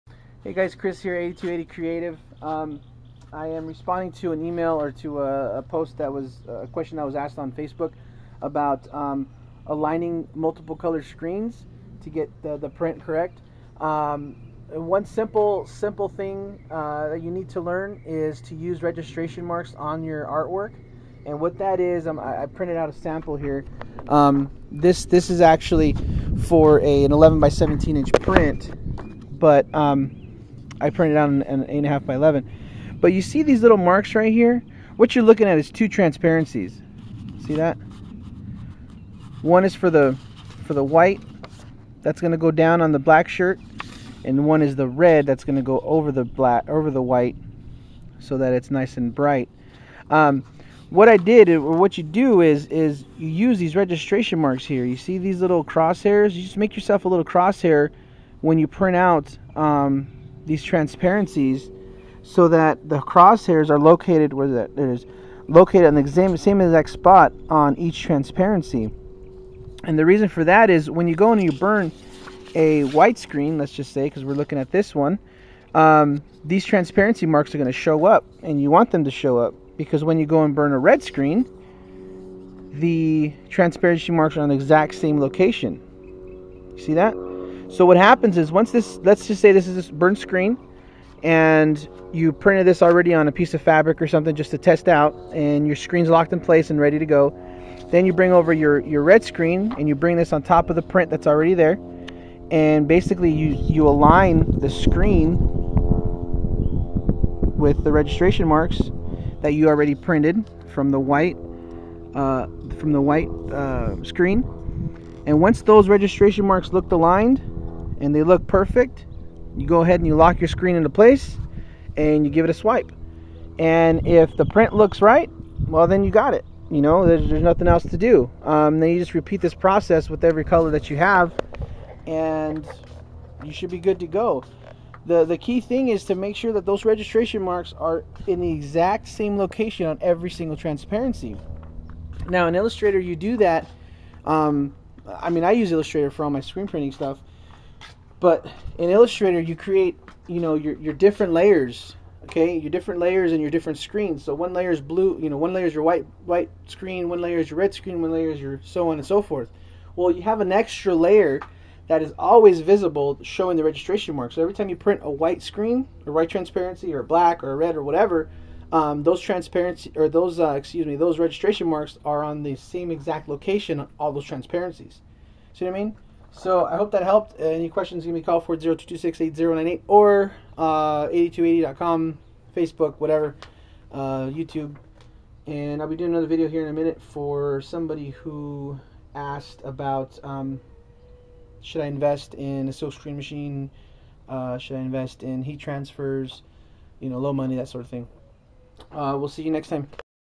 82eighty.com | aligning screens and transparencies
Fernando is having trouble aligning screens. I talk briefly about registration marks on your transparencies and screens..